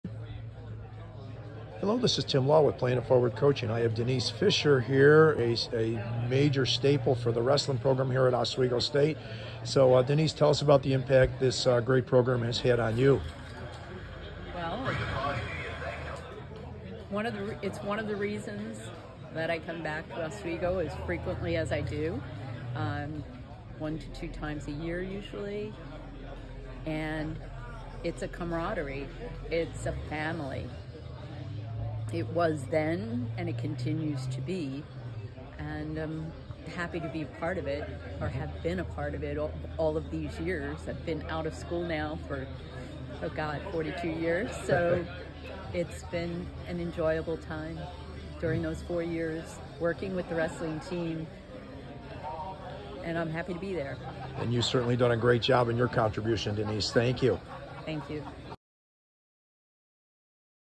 Oswego wrestling manager, extraordinaire Denise Fischer, shares her Oswego story
Many times the incredible support team of a great program can be overlooked. Not so with Denise Fischer, a staple of going above and beyond a few decades ago with Laker Wrestling!

She was an amazing part of some excellent wrestling teams that she assisted with and was the right hand person of the coaching staff doing many of the little things that make a good team better.

Tim